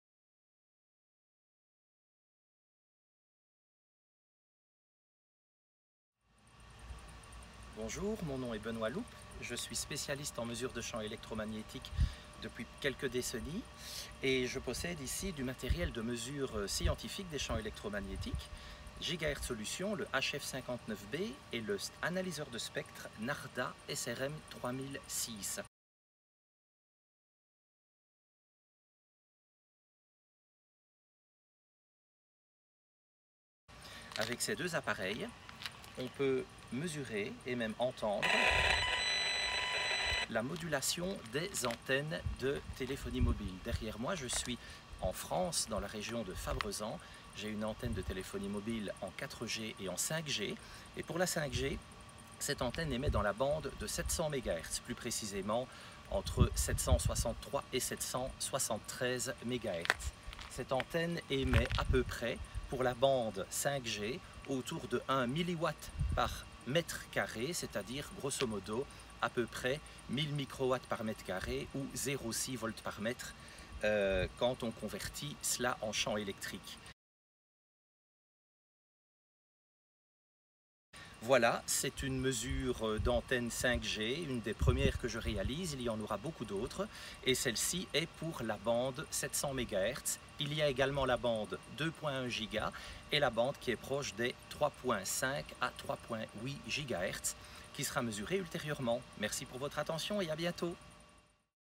5G, ondes et santé - Mesure antenne relais 5G - 700 MHz par Benoît Louppe
Mesure au pied d'une antenne relais 5G - -bande 700 MHz - région de Fabrezan (France) - Août 2021 - Matériel utilisé: HF59B de Gigahertz-Solutions et analyseur de spectre SRM-3006 - Mesure effectuée par Benoît Louppe, consultant indépendant en mesure de champs électromagnétiques . Intensité mesurée de +- 0,6 V/m (+- 1mW/m2).
Pour en savoir plus, pour mesurer et se protéger, visitez le